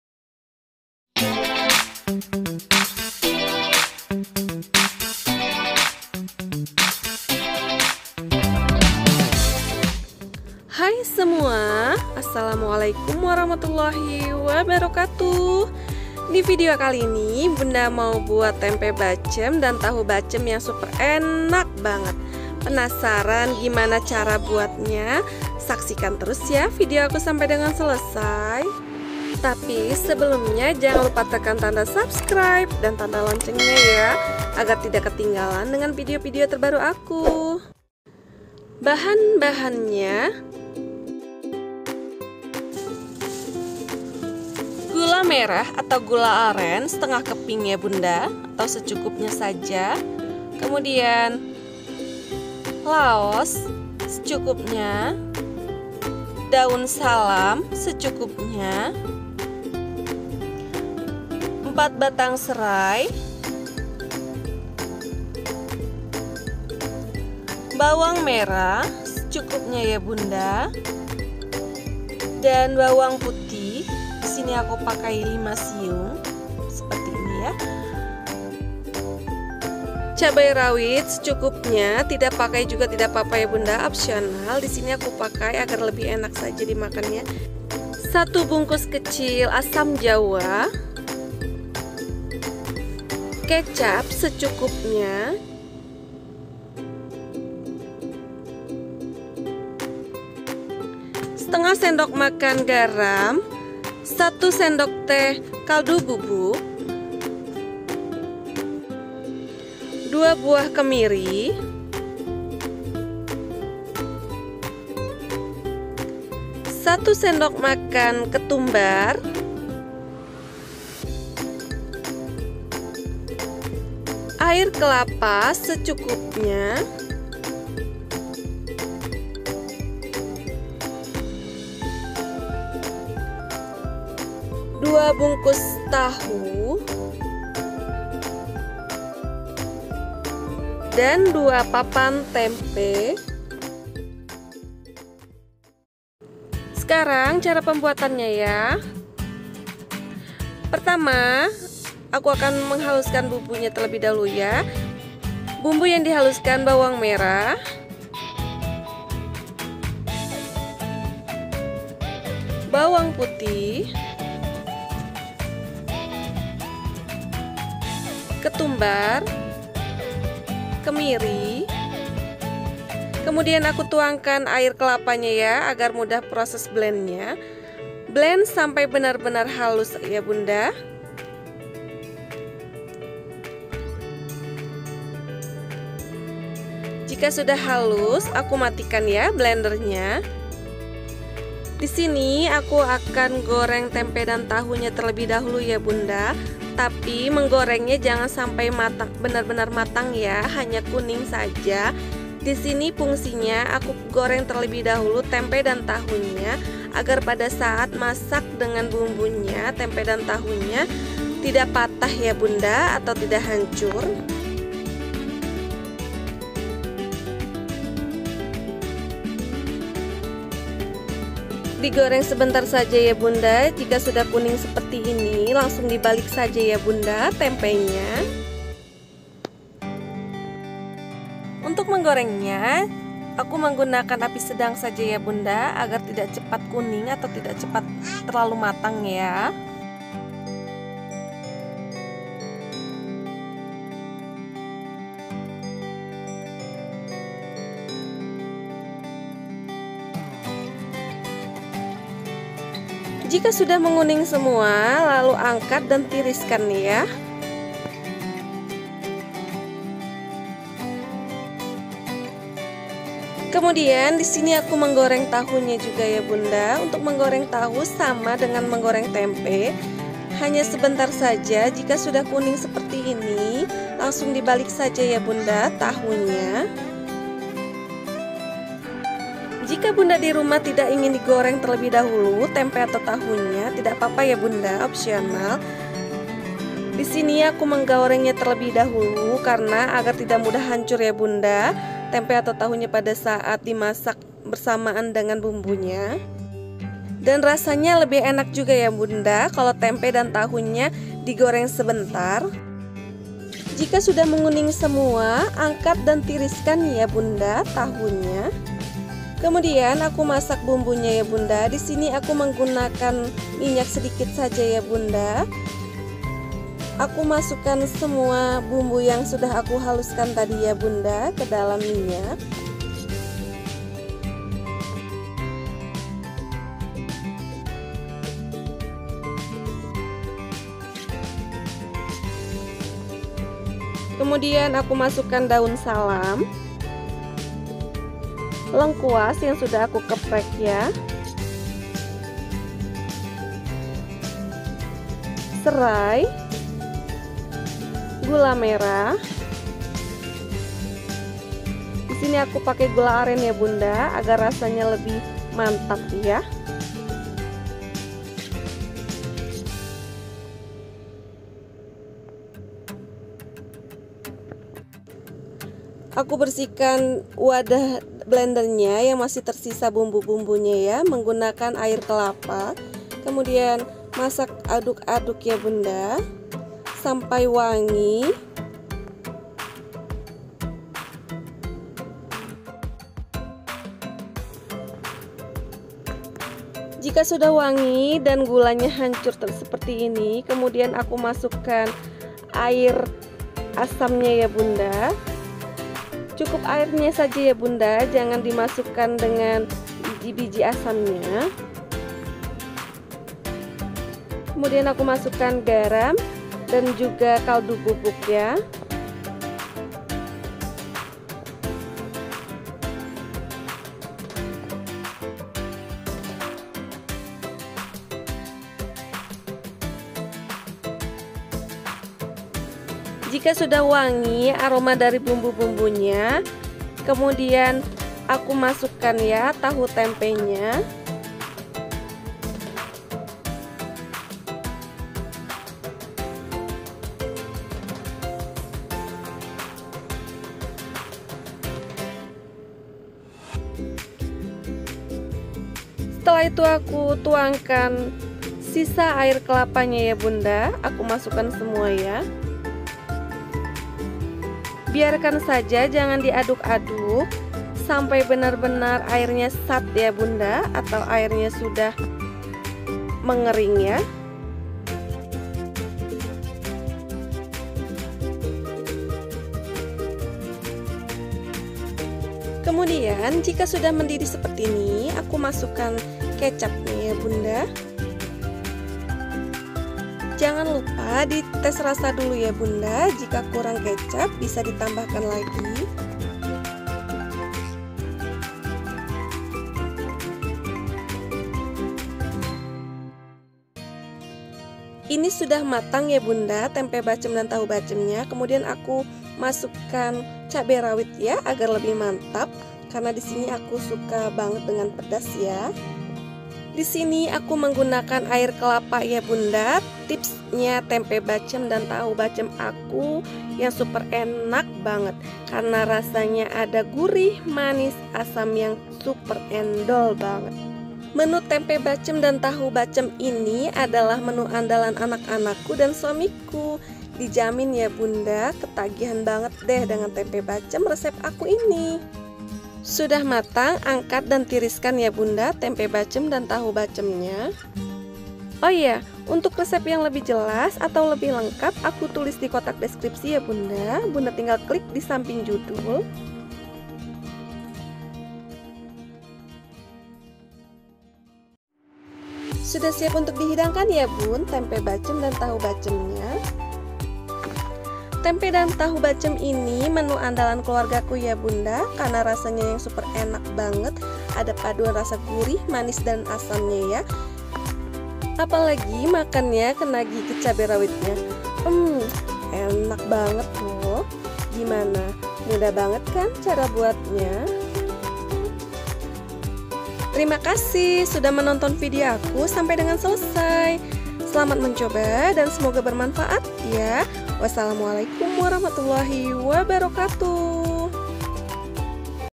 Rahasia Resep Buat Tempe Tahu Bacem super Enak pakai air kelapa🤤
Resep Tempe Tahu Bacem

Bahan - bahan :

√ tempe 2 papan
√ Tahu 2 bungkus
√ gula merah 1/2 keping atau secukupnya
√ garam 1/2 SDM
√ kaldu bubuk 1 sdt
√ air kelapa secukupnya
√ cabai rawit secukupnya (optional)
√ kemiri 2 buah
√ ketumbar 1 SDM
√ bawang merah 15 buah atau secukupnya
√ bawang putih 5 buah atau secukupnya
√ serai 4 batang
√ lengkuas 1 ruas
√ daun salam
√ asam Jawa 1 bungkus kecil